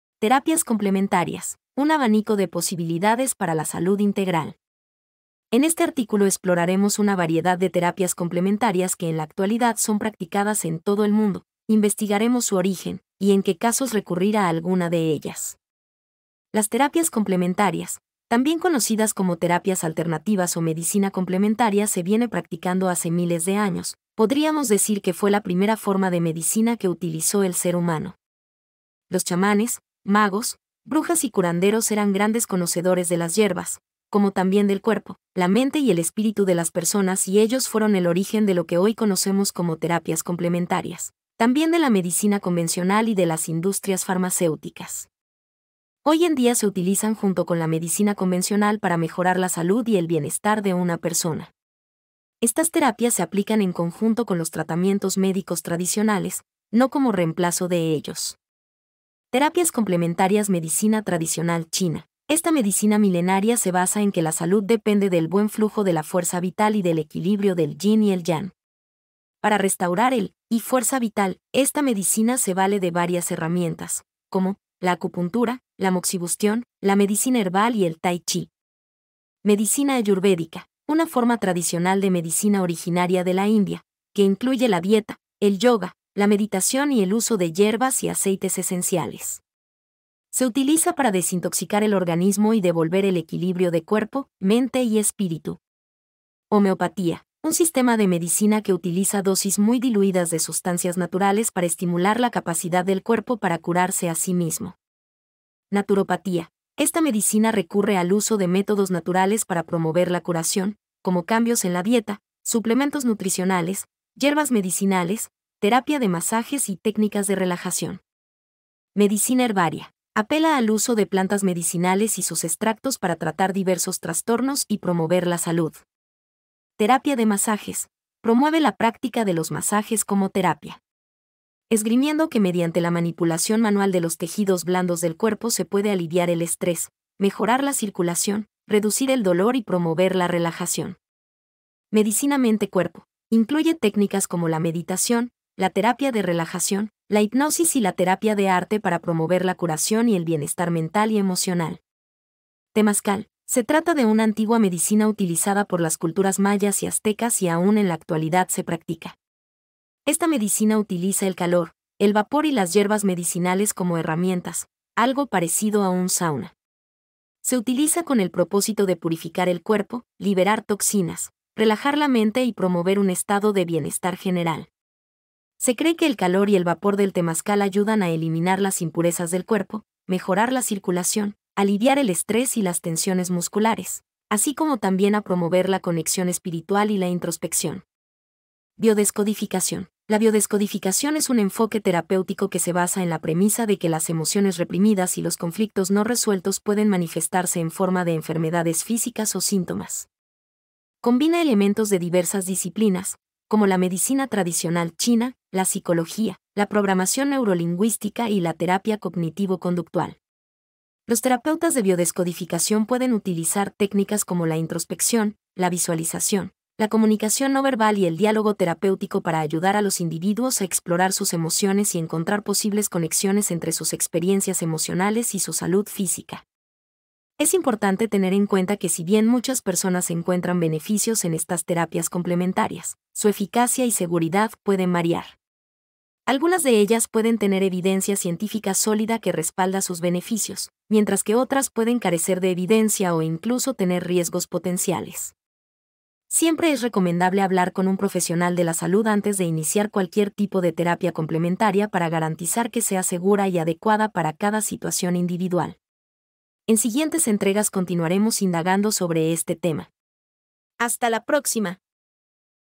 Terapias complementarias: Un abanico de posibilidades para la salud integral.
En este video exploraremos una variedad de terapias complementarias que en la actualidad son practicadas en todo el mundo, investigaremos su origen, y en qué casos recurrir  a alguna de ellas.